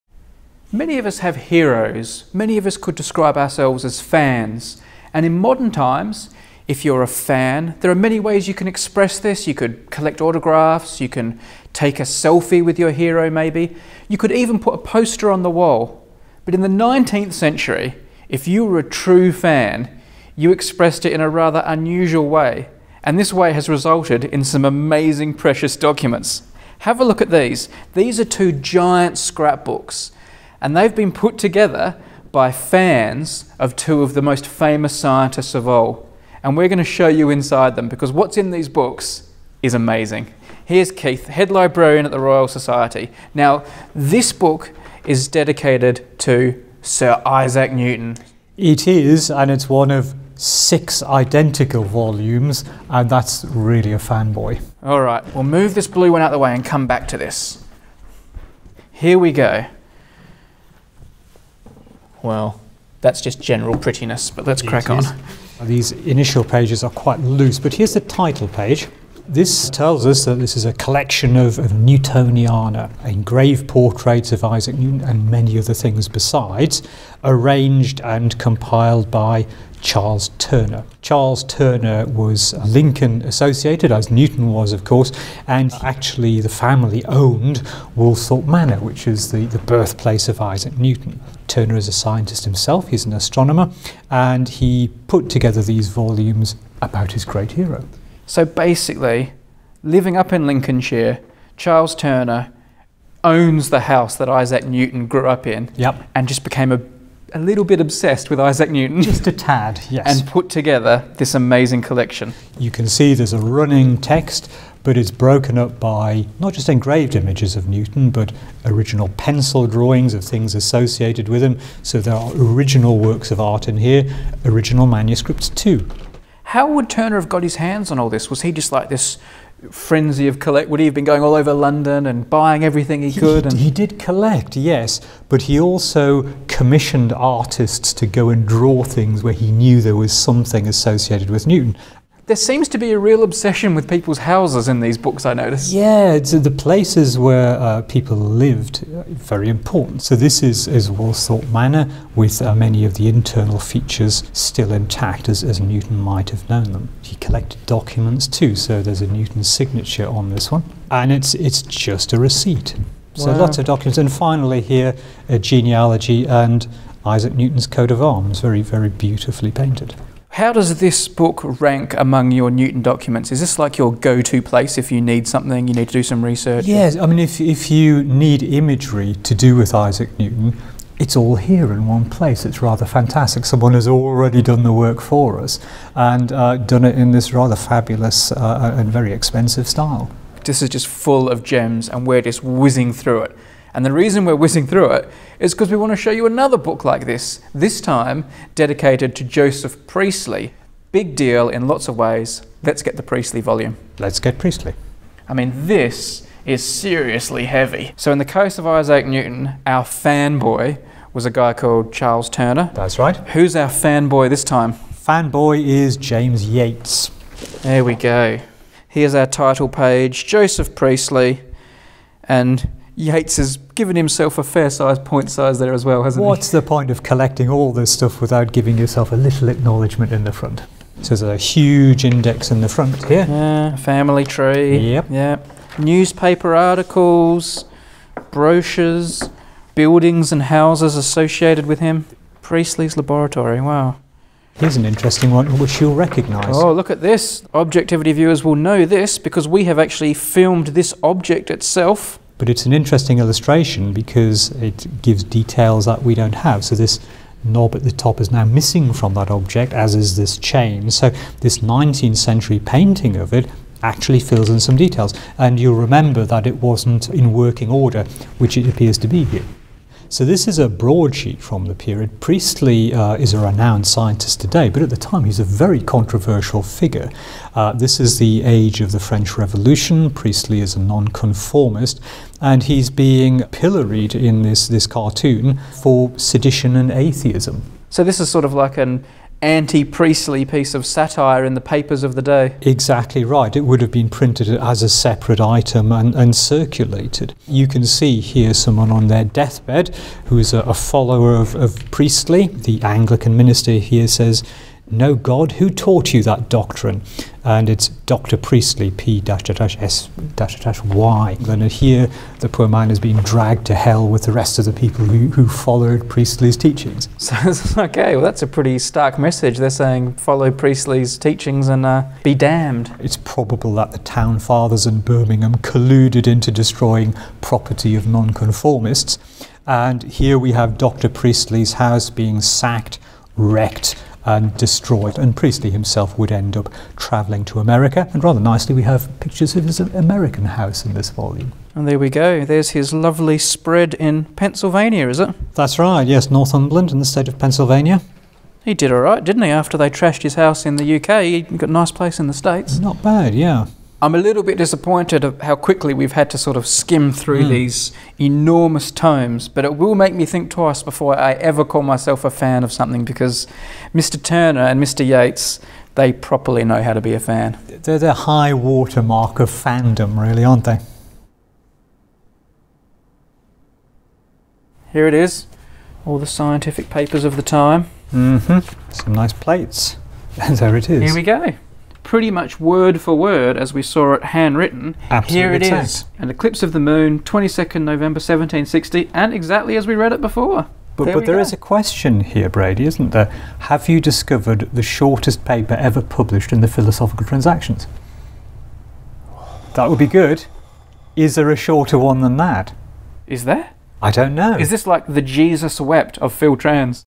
Giant Fan Books - Objectivity 29
Brady joins Keith at The Royal Society to look at two very special fan books based on Sir Isaac Newton and Joseph Priestley.